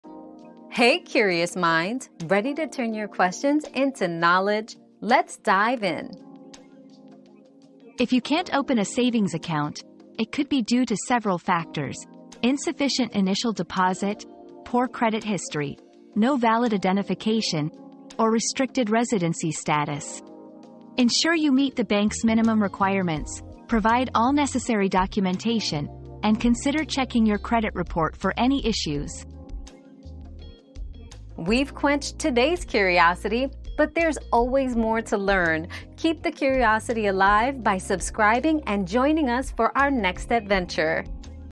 Why can't I open a savings account?
Unlocking Savings: Overcoming Account Opening Challenges 👉 Savings Account Woes 👉 Discover why you may be facing hurdles in opening a savings account and learn how to overcome them with our helpful tips.

Laura S. Harris (2024, May 22.) Why can't I open a savings account?
    🌐 AskAbout.video